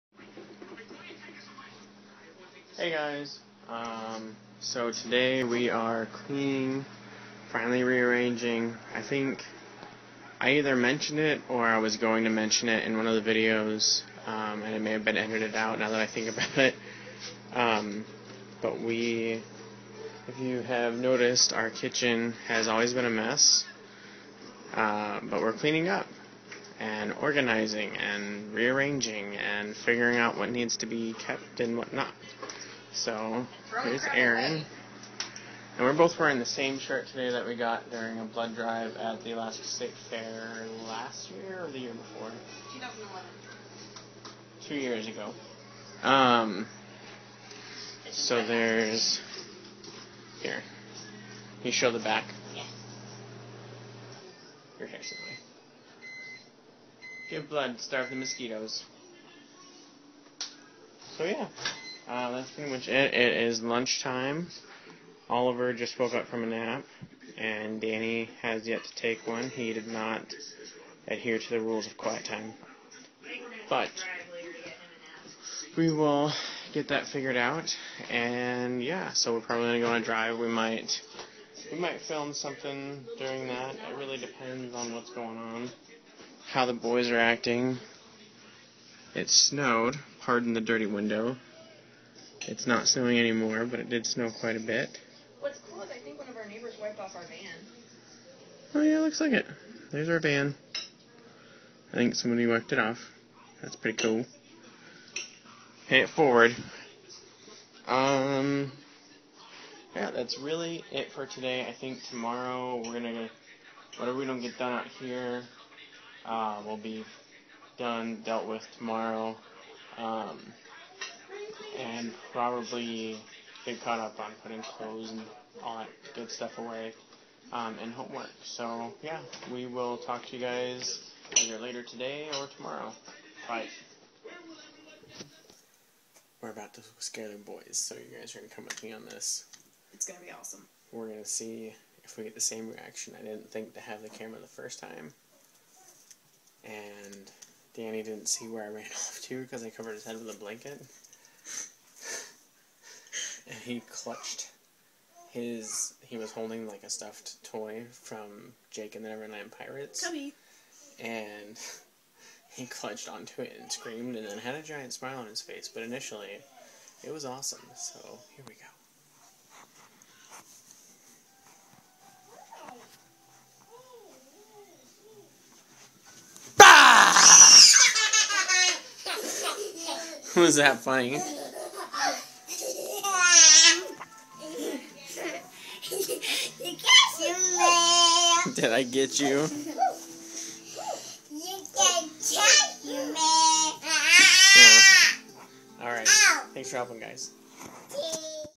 Cleaning House! [Day 7 ~ 03/23/2013]
Cleaning and organizing our apartment! Scaring my kids (I'm a terrific father :))